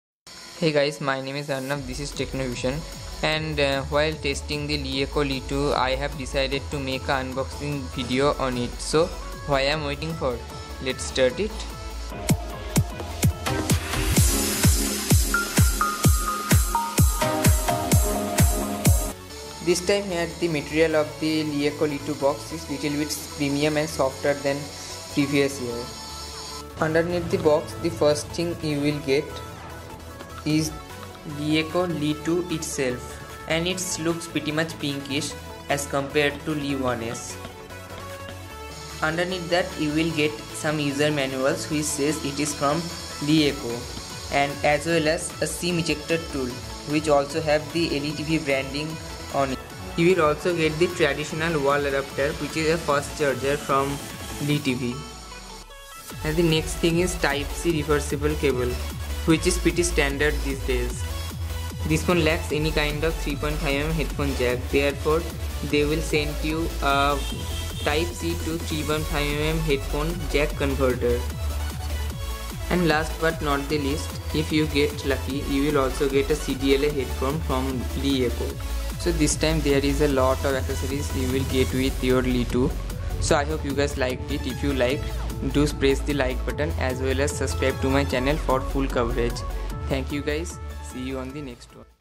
LeEco Le 2 Quick Unboxing (Indian Variant)
This is a quick unboxing of LeEco Le 2 Snapdragon Variant, so without wasting any time lets enjoy it..!